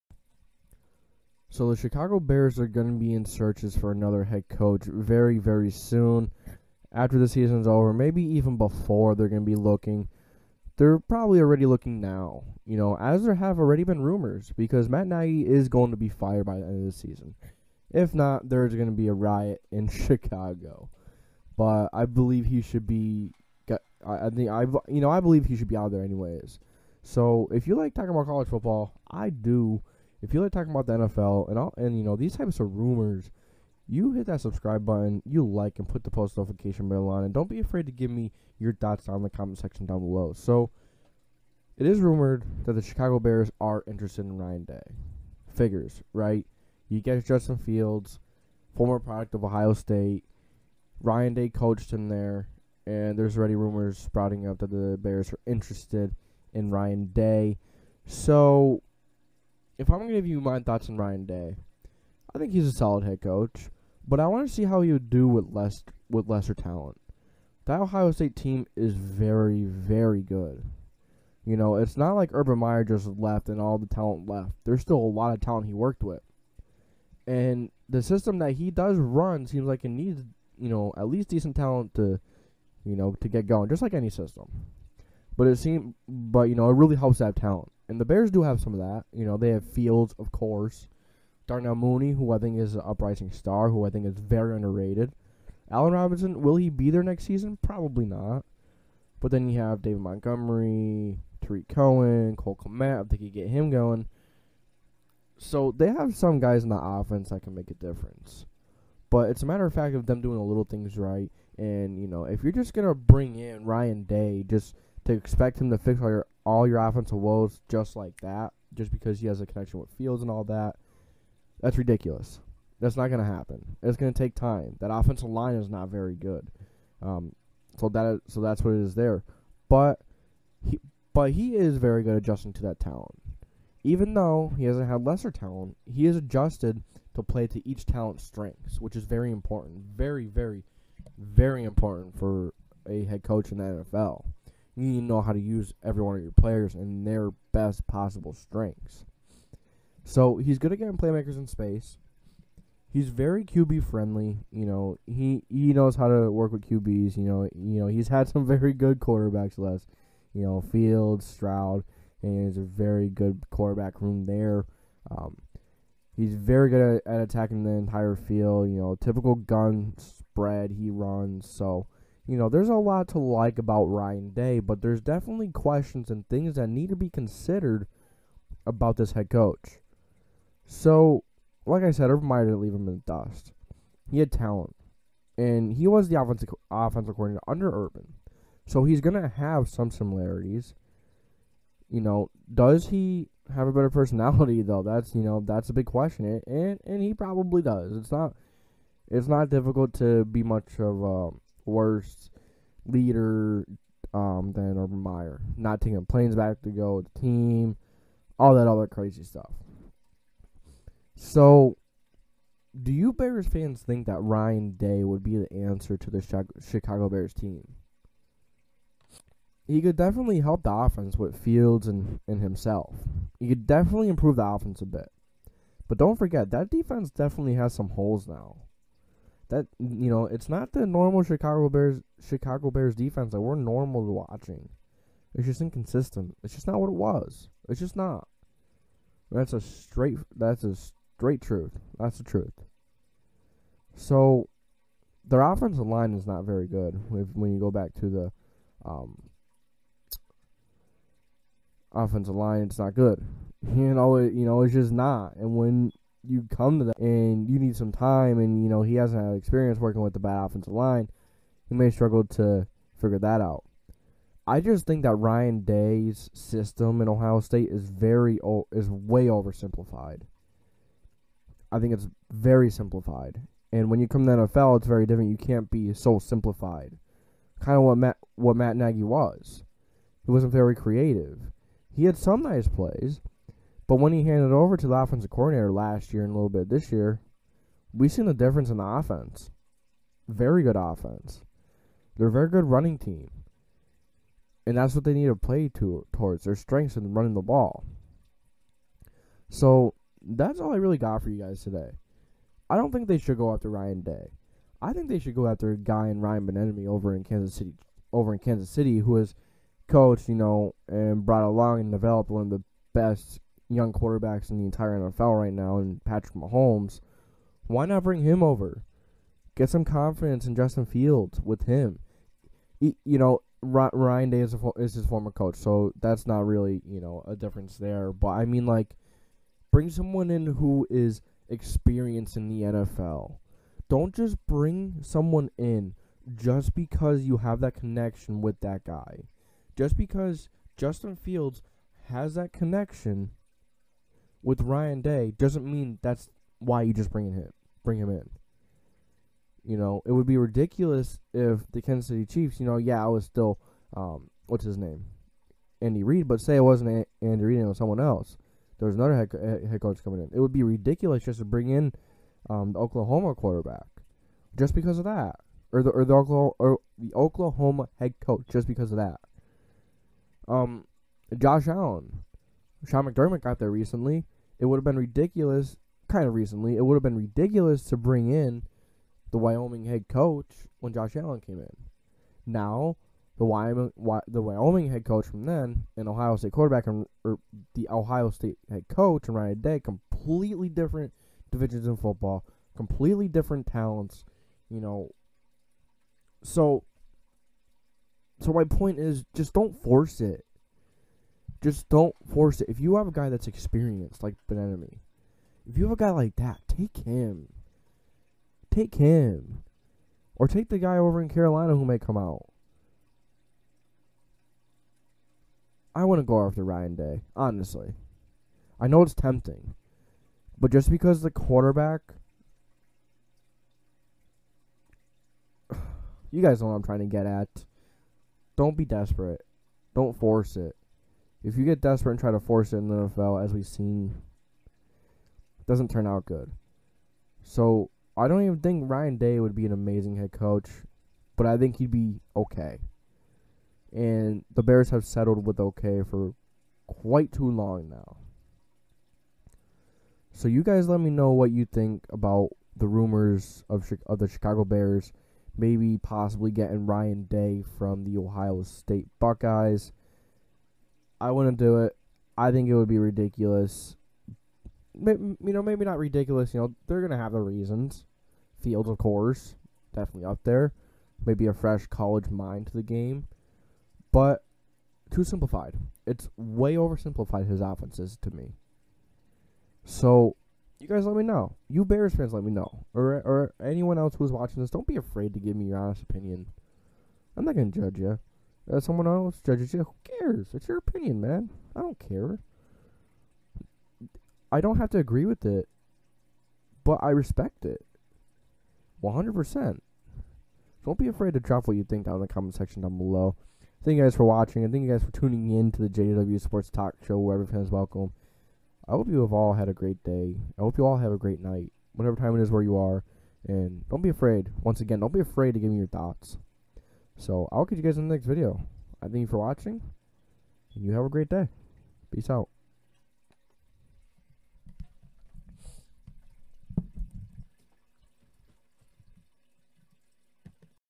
should Ryan Day be the next Chicago Bears head coach? Matt Nagy, Justin fields
The Chicago Bears are getting ready to move on from head coach Matt Nagy after being stuck in neutral and beginning to go in reverse. Ryan Day, Justin Fields' former head coach from Ohio state has come up as a potential coach candidate for the Bears head coaching job. Should the Bears go after Ryan Day, or maybe a more NFL ready/experienced coach like Eric Bieniemy who helped develop one of the best young quarterbacks in the NFL.